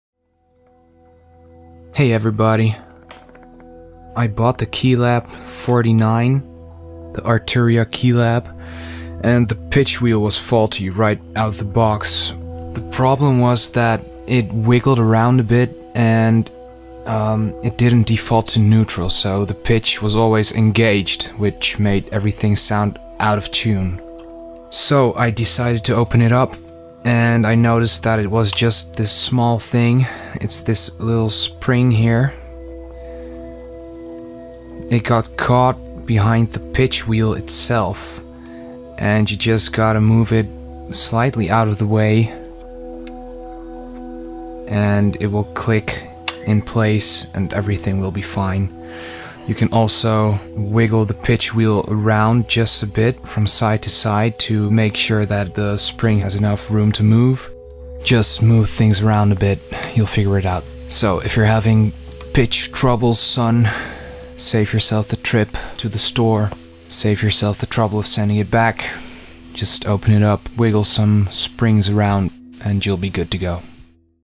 Arturia Keylab Pitchwheel Fix
Arturia Keylabs are very nice pieces of hardware, but are known to have issues right out of the box. In this video I address the faulty pitch wheel: The pitch MIDI message won't default to 64, but wiggle around and make everything sound out of tune.
You can physically feel that the wheel is unstable and you can see the MIDI messages it sends are different from 64.